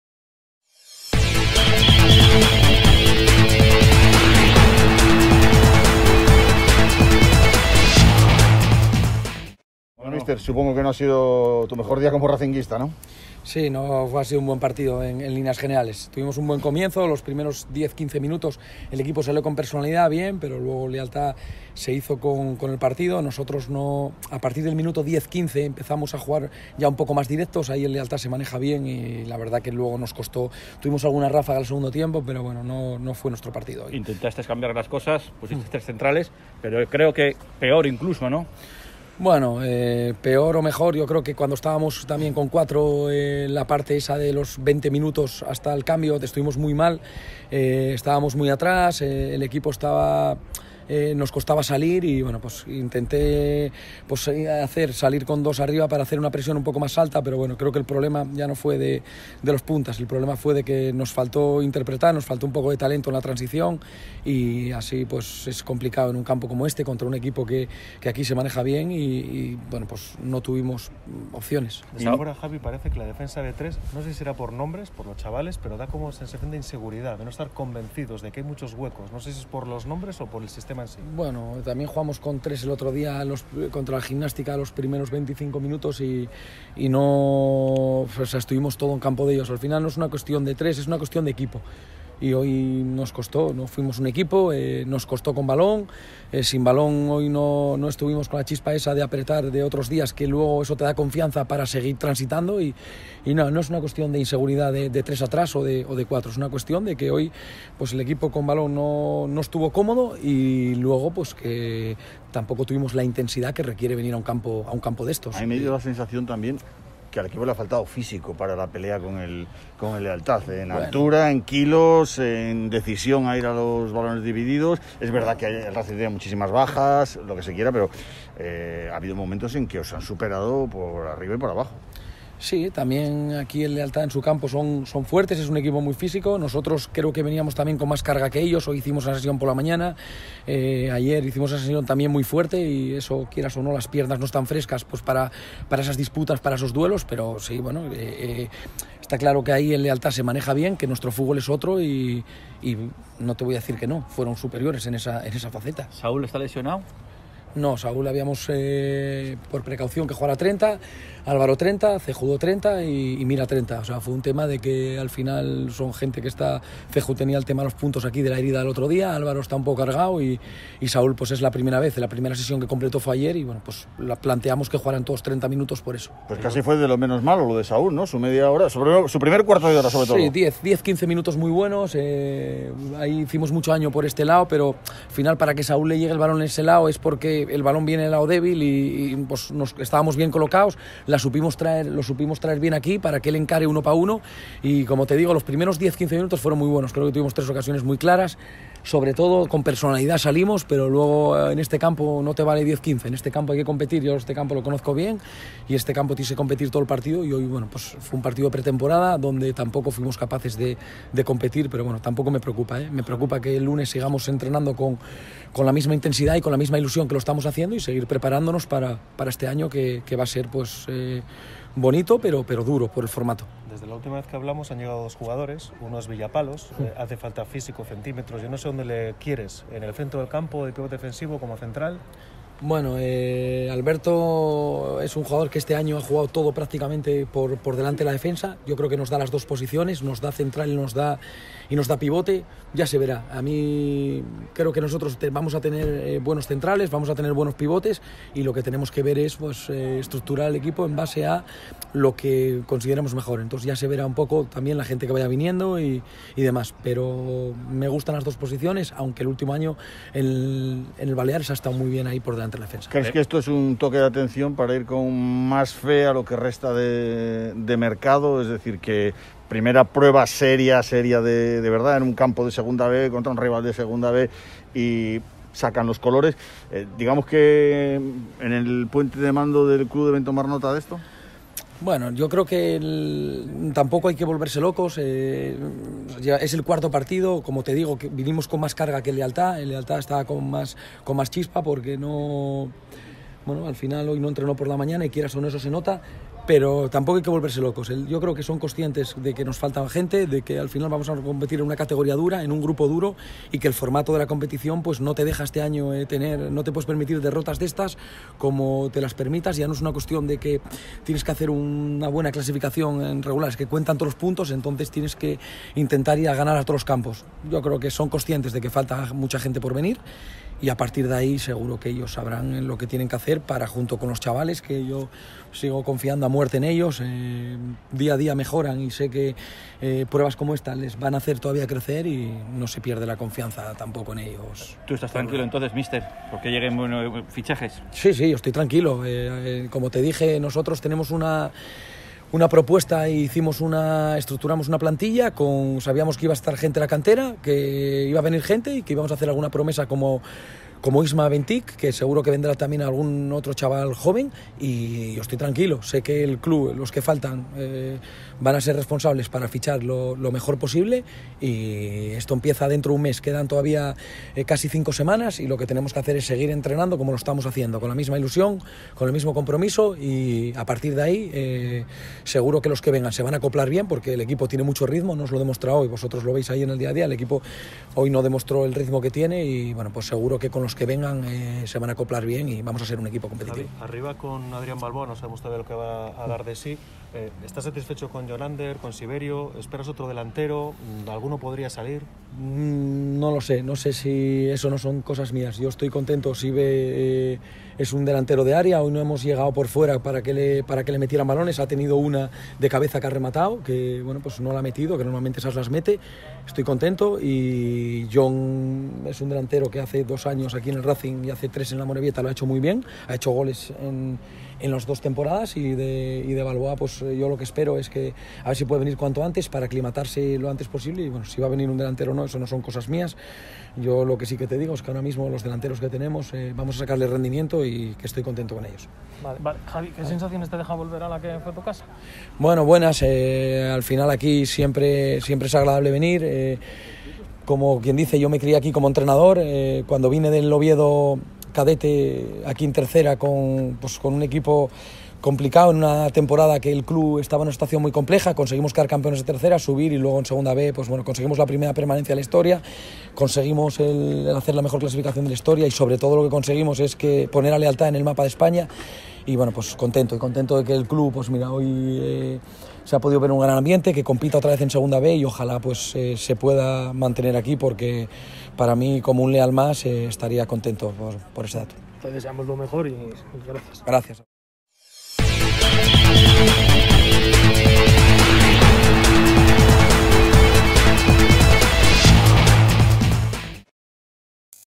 LEALTAD 1 RÁCING DE SANTANDER 0 JAVIER ROZADA
Declaraciones del entrenador del Rácing de Santander al final del partido.